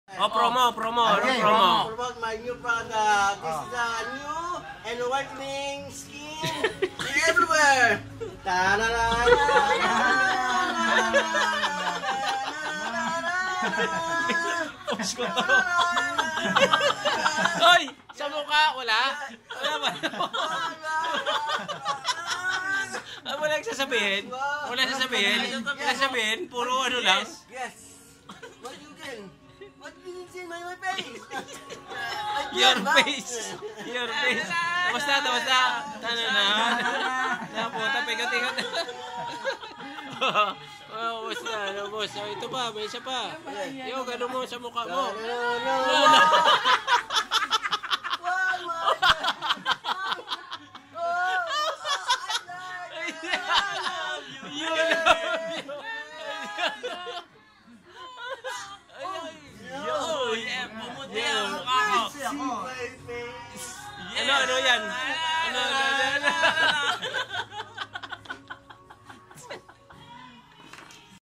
Boy Bangs Show
Ang lupet sumayaw ni boy bangs muntik na malunok ang ???watch till the end hahahaha